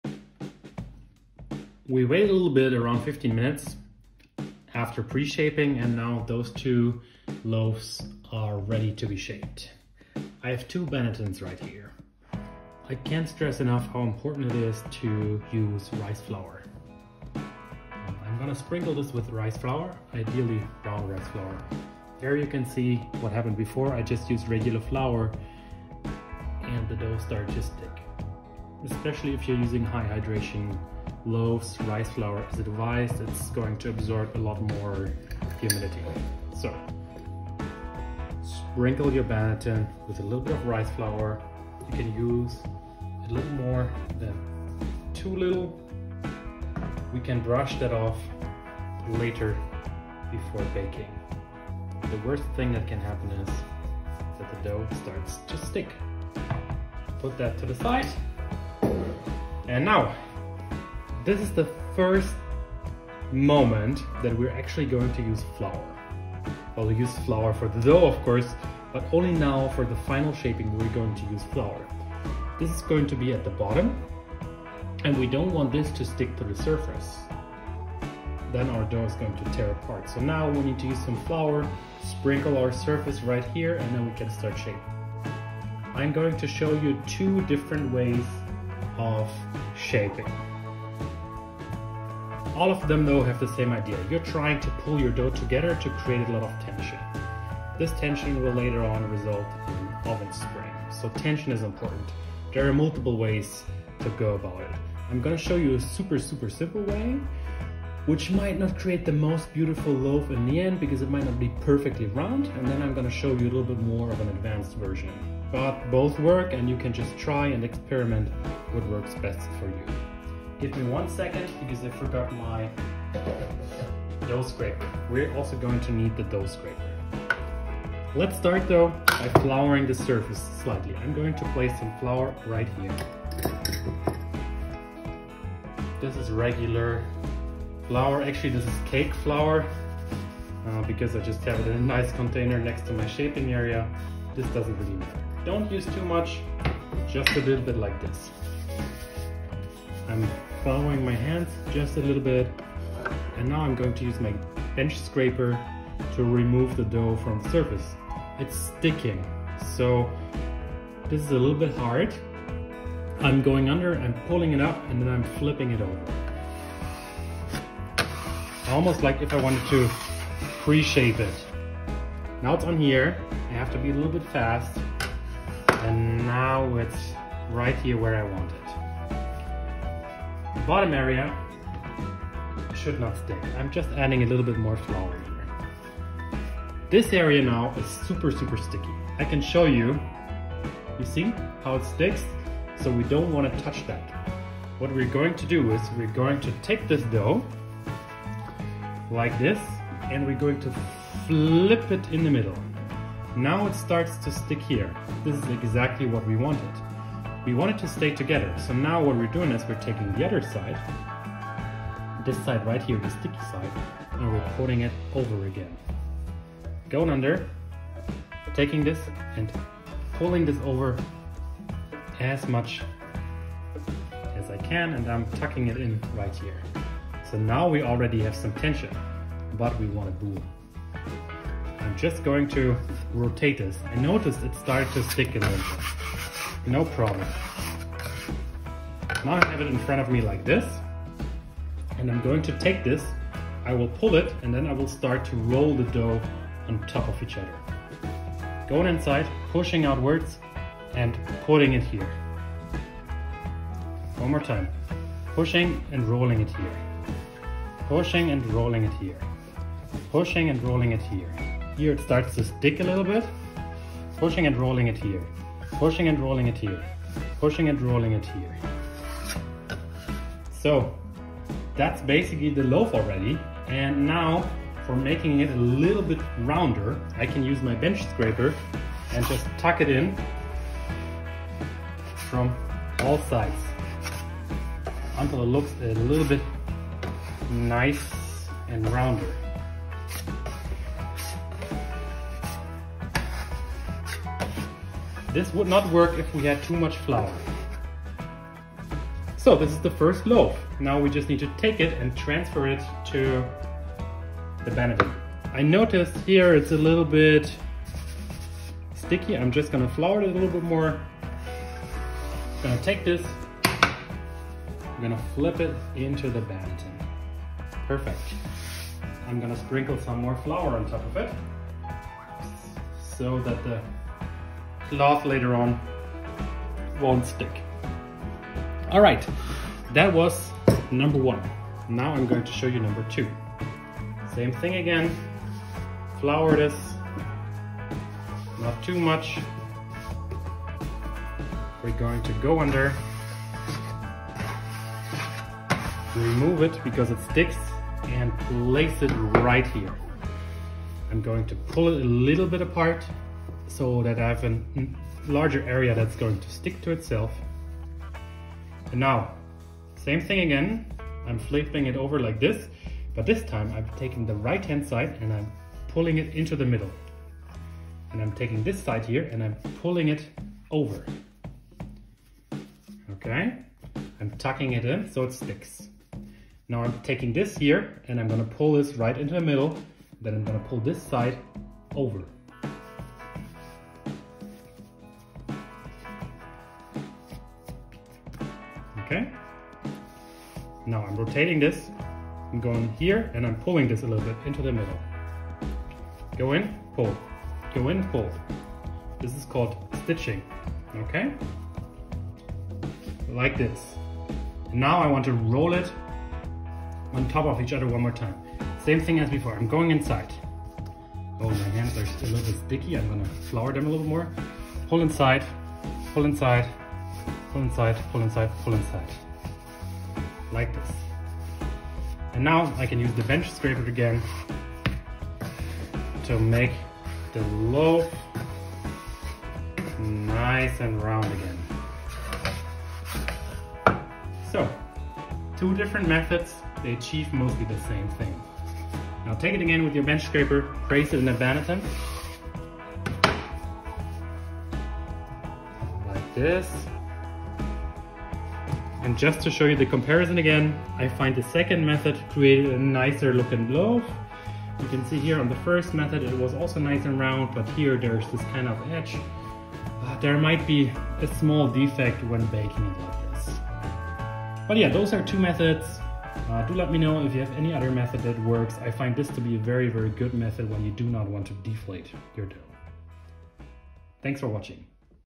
The ONLY SOURDOUGH BREAD SHAPING Tutorial You Will Ever Need
This is the most complete tutorial on how you shape sourdough bread, step by step so that you can follow easily. You will mater 2 techniques - beginner and then advanced. The methods also work for yeast based doughs.

So what exactly is shaping and when is it done? After pre-shaping or directly after bulk fermenting you want to shape your bread into the final form it should have after the bake.  A very tight shaping means that your dough holds together very well and thus it will be able to hold its shape during the baking stage. You will likely have more oven spring. You are basically glueing the dough together with each movement of your hands.

Only during shaping stage you want to add some flour to your surface. For pre-shaping no flour is needed. Flip your dough upside down and then follow the instructions in this video. Be very gentle with your dough all the time. You want to use as little water as possible.

If you want to shape a batard, simply go with method one. Do not tuck the dough together like I did in the end.

Now if this does not work it could be that you over-fermented your dough, or you used too much water for your flour. Many reasons. This happens to me to and what I then do is, I just take the dough and toss it into a loaf pan. That saved me many breads.

Side note - when making pizza, this is in excellent way to shape your dough into pizza balls. I like to do it this way, since the method is very gentle, making sure your pizza is not degassed.

Useful videos:
Debaked ep. 1 - Pizza journey to Napoli
Debaked ep. 2 - Journey to a flour mill

Chapters:
0:00 Intro
0:14 Preparing the Banneton with rice flour
2:40 Easy technique
5:40 Advanced technique
7:06 Tightening the dough